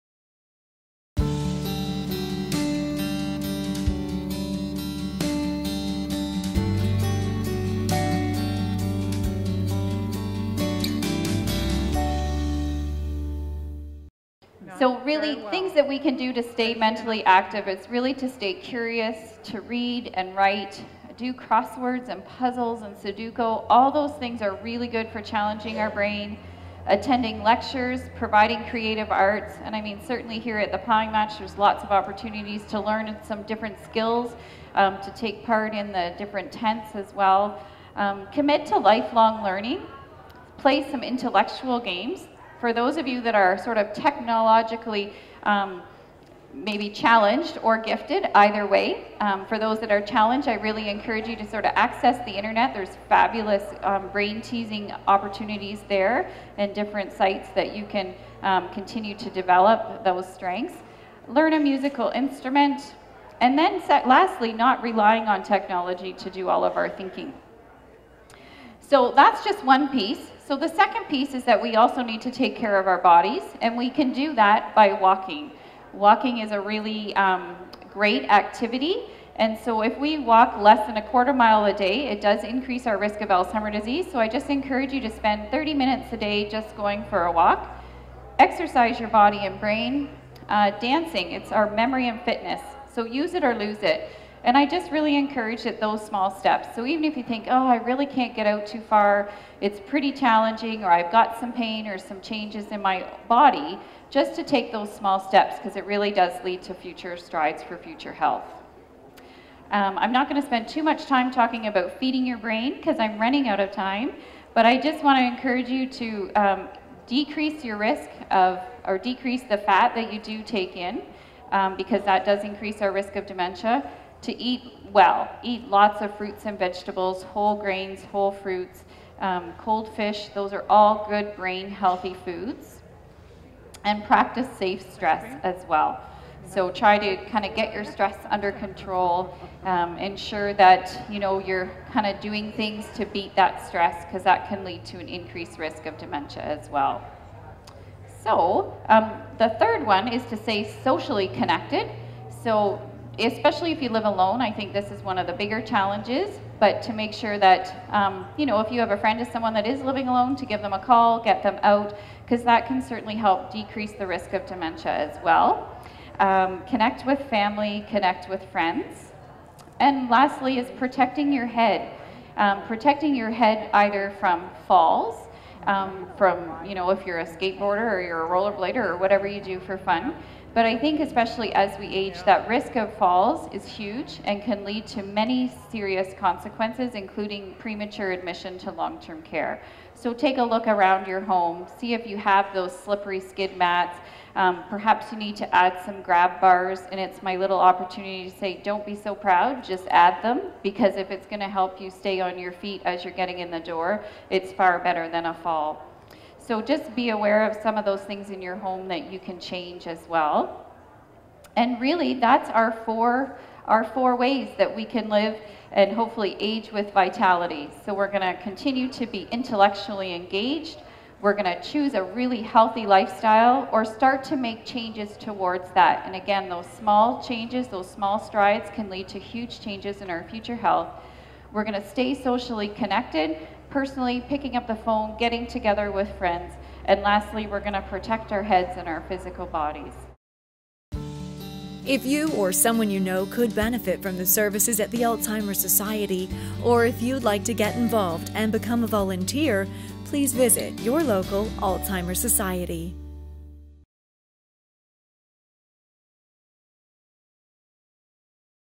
Alzheimer Awareness - Aging with Vitality: Stay Mentally Active / Work Your Body / Feed Your Brain
Learn more on how to Age with Vitality

It's important to stay mentally active:
- Stay curious
- Read & Write
- Do crosswords, puzzles, sudoku
- Attend lectures, creative arts
- Commit to life-long learning
- Play intellectual games
- Learn a musical instrument
- Don't rely on technology to do your thinking